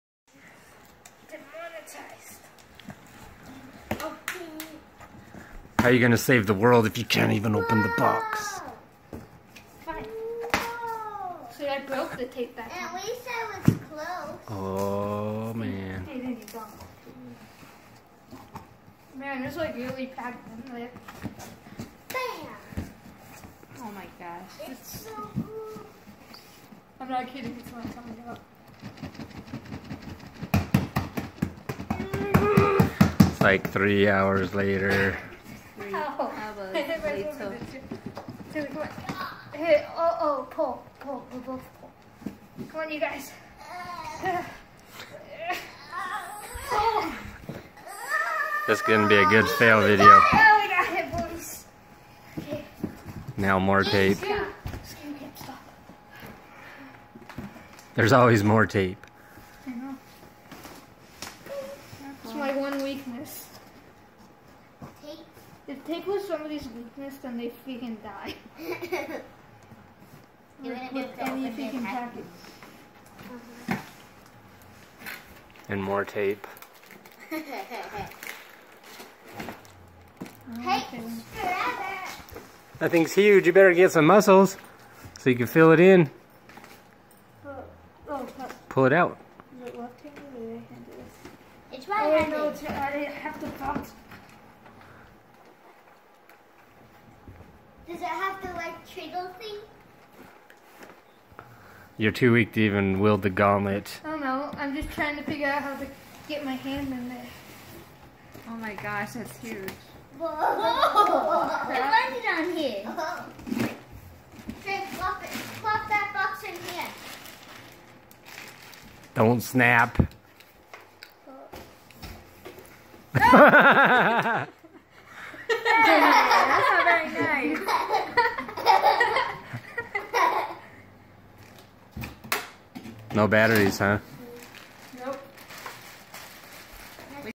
Avengers Endgame Marvel Legends Power Gauntlet Unboxing - Tristan
The long awaited Power Gauntlet has arrived and no batteries included!